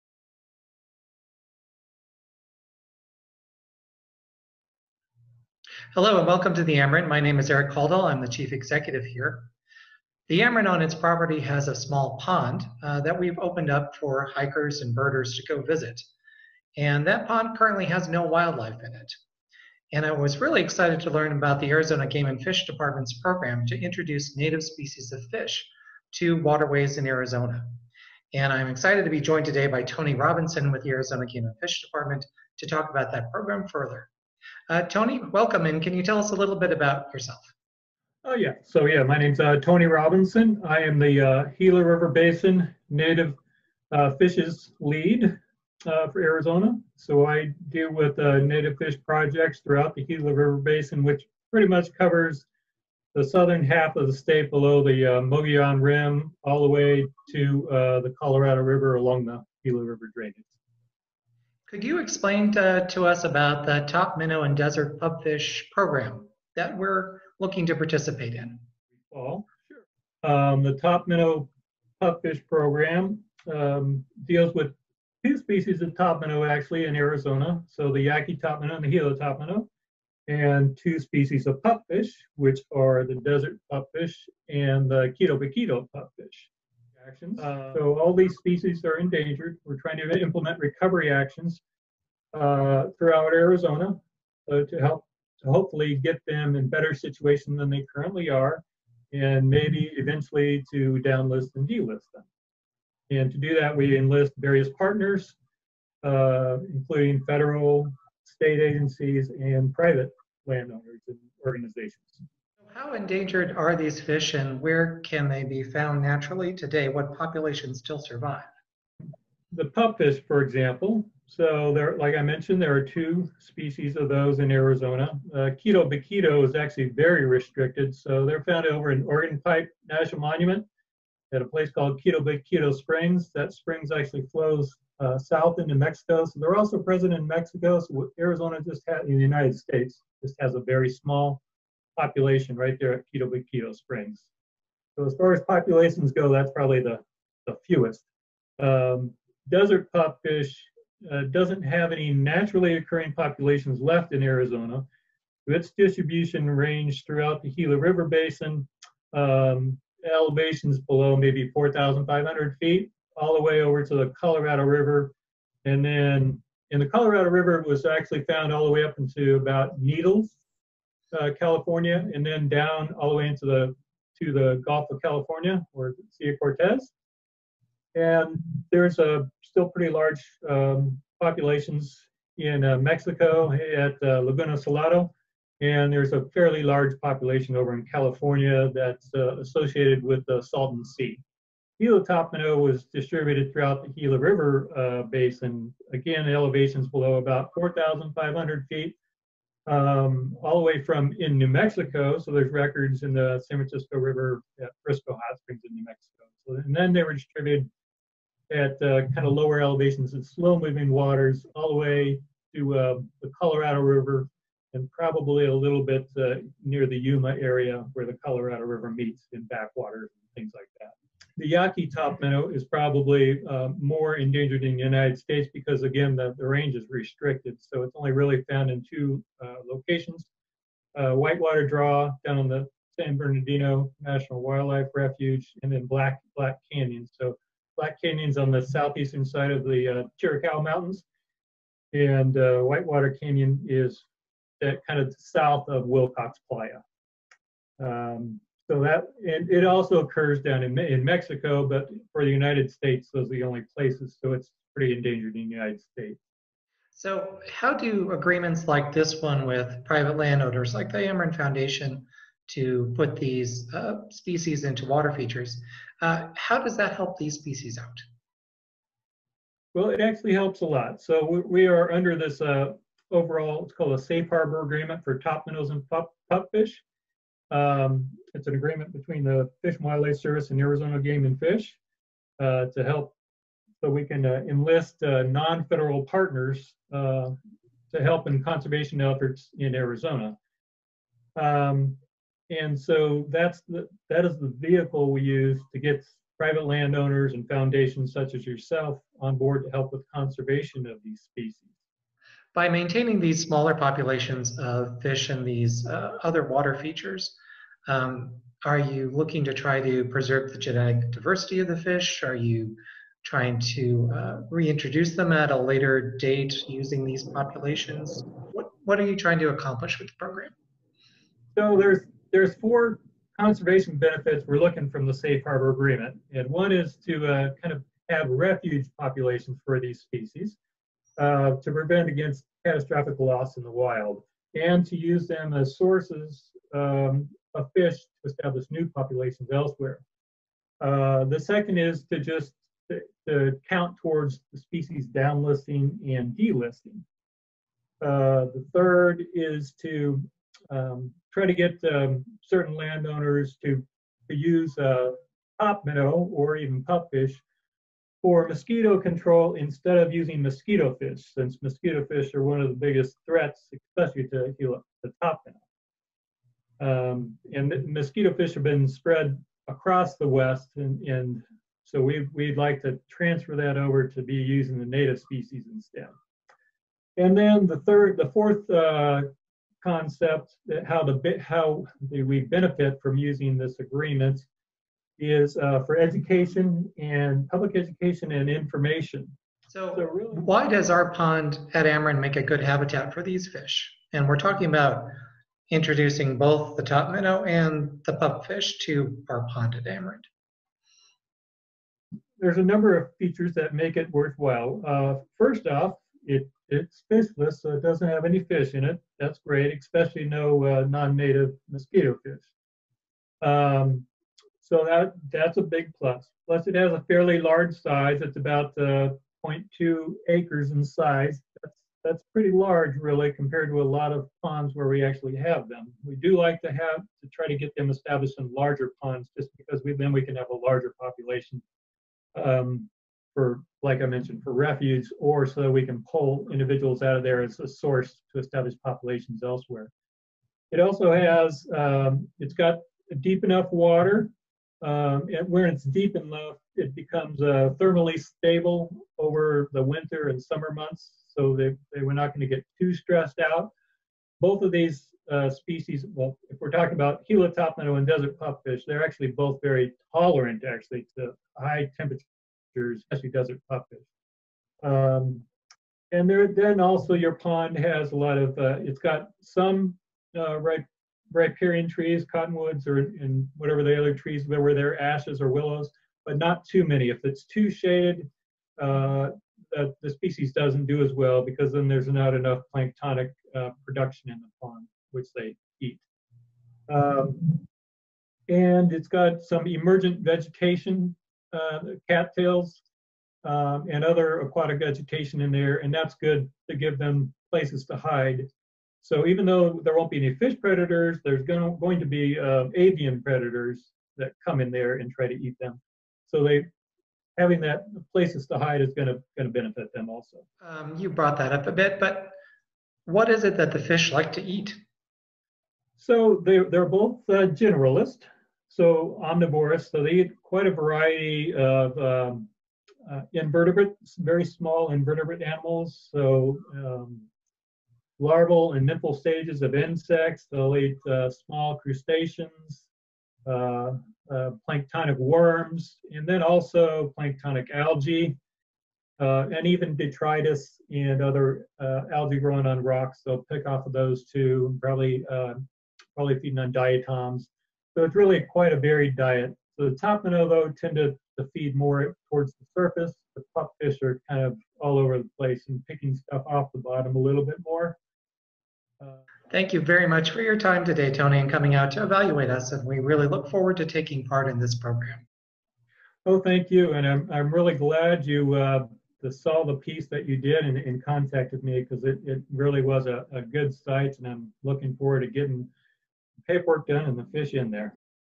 Endangered Fish find a Safe Harbor at Amerind
The Arizona Game and Fish Department will be adding two endangered fish species to Amerind's bird pond this summer: the Gila Topminnow and the Desert Pupfish. These fish once called our region's rivers and streams home. Today many of the waterways they used to inhabit are no long available to them.

Want to learn more? In this week's video, Tony Robinson from Arizona Game and Fish has a conversation with Eric Kaldahl about the program and why Amerind's bird pond will make a great home for these Arizona species.

These native fish are great mosquito eaters, too! If you are interested in this program yourself, learn more by visiting the Arizona Game and Fish Department